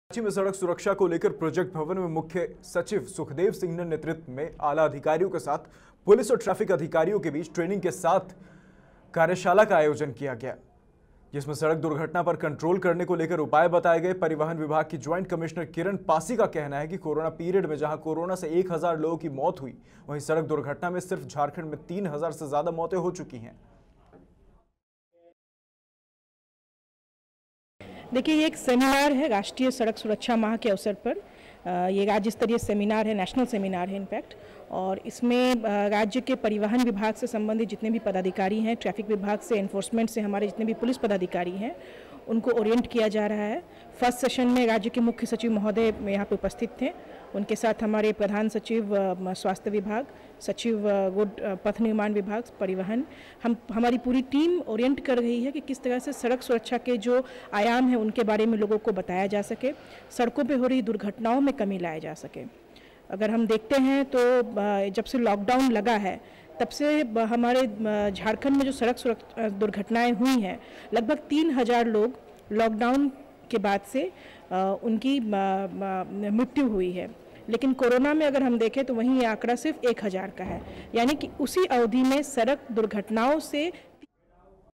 Ranchi : सीएस सुखदेव सिंह के नेतृत्व में सड़क सुरक्षा को लेकर कार्यशाला का आयोजन, आधिकारियों को...
रांची : सीएस सुखदेव सिंह के नेतृत्व में सड़क सुरक्षा को लेकर कार्यशाला का आयोजन, आधिकारियों को...